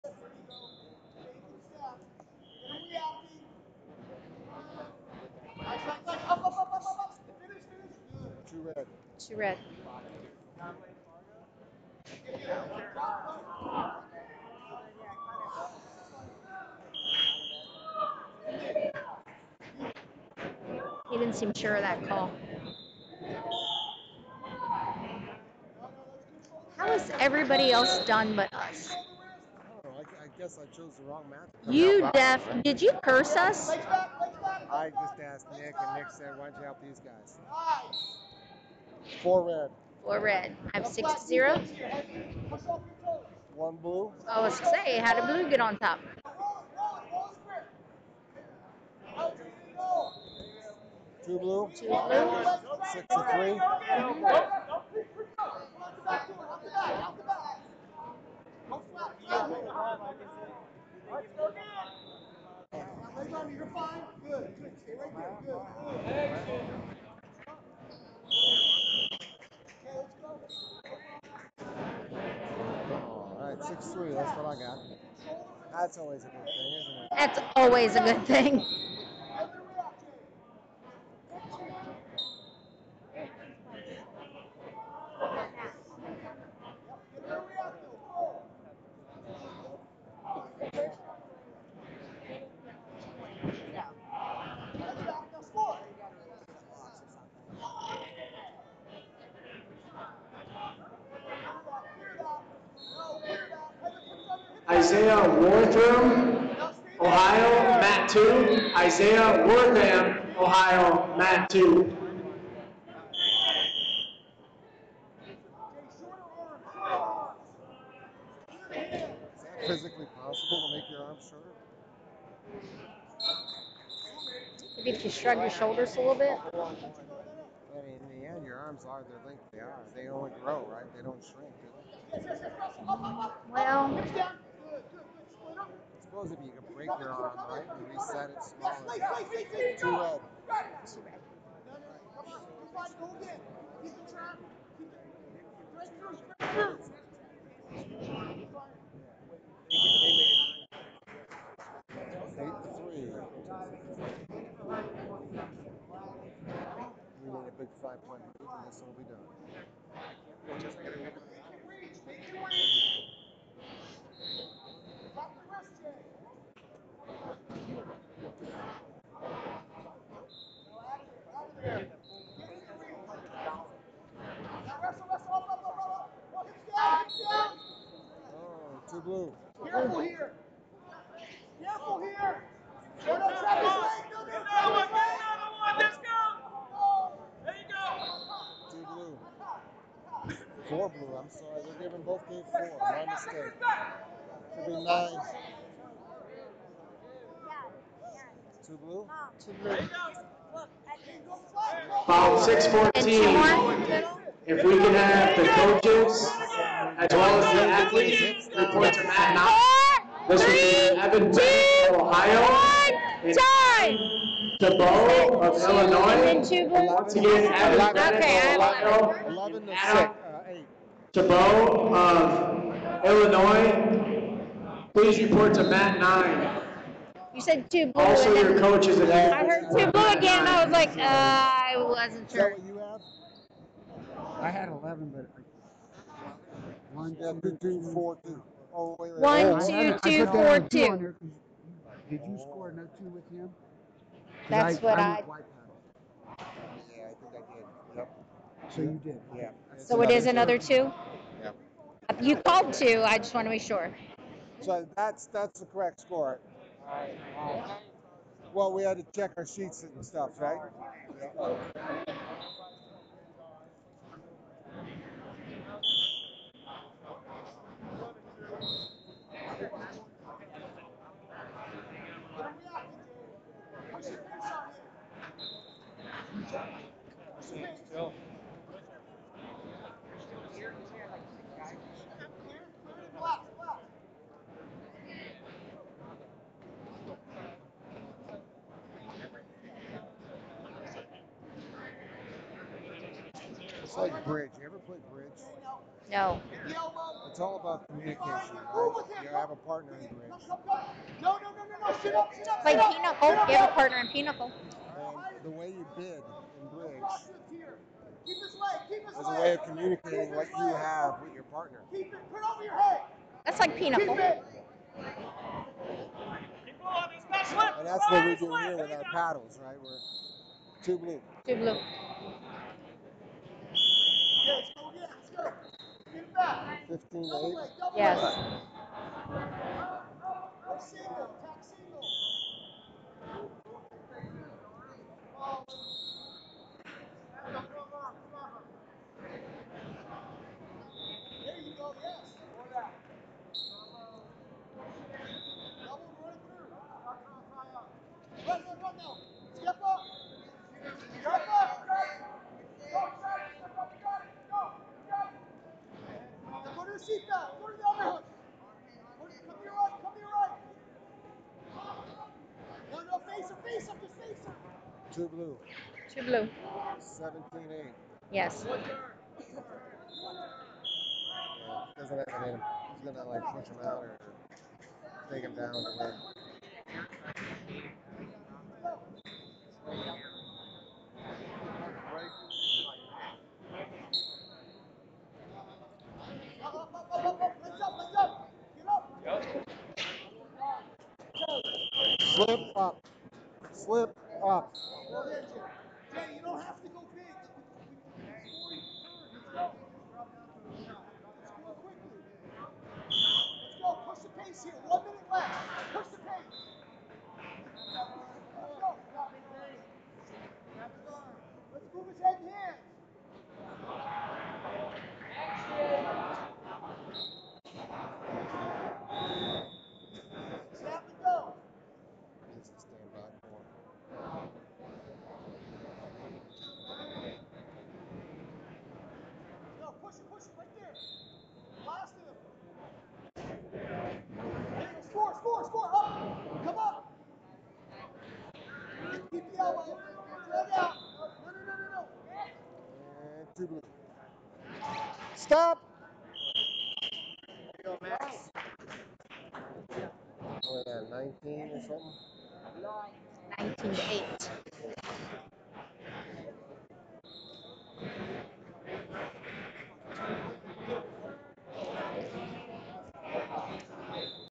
Junior Men 195 Jadon Martin California Vs Zane Lanham West Virginia
2019 Central Regional - Junior Men 195 Jadon Martin California Vs Zane Lanham West Virginia